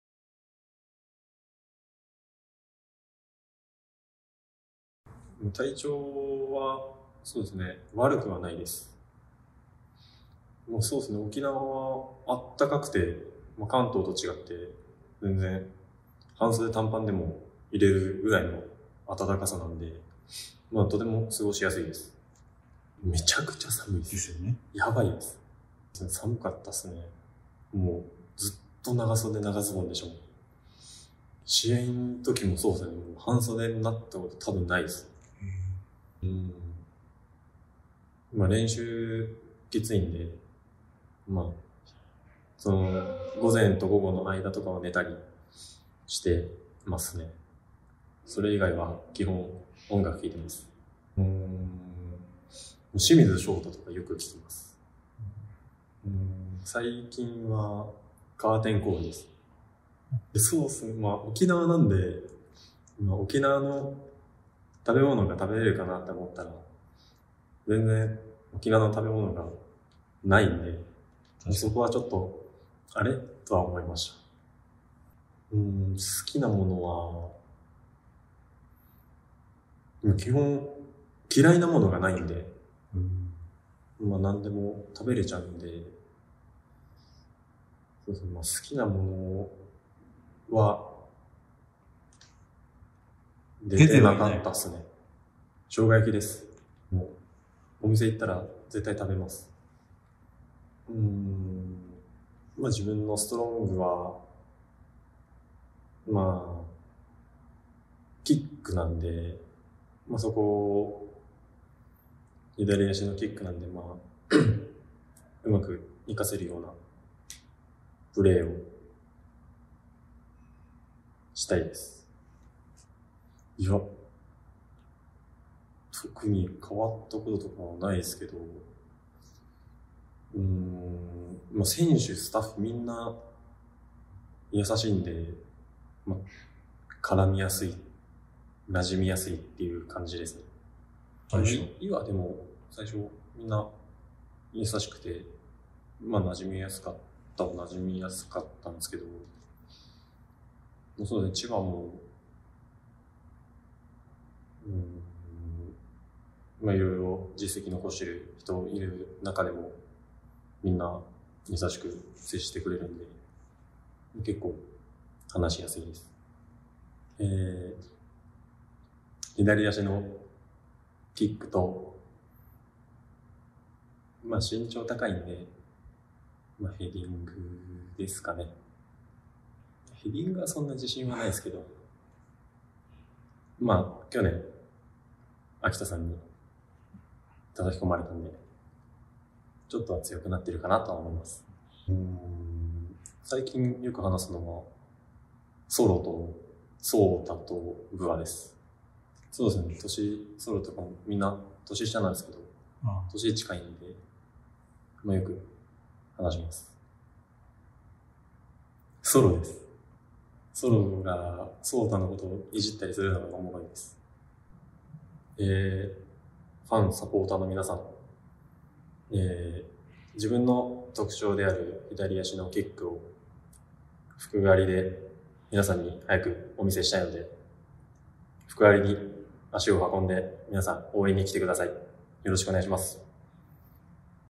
【ジェフ新加入】佐々木 翔悟選手インタビュー（取材日：沖縄キャンプ期間中）
2/19（土）14:00K/O vs いわてグルージャ盛岡（フクダ電子アリーナ）の試合情報はこちら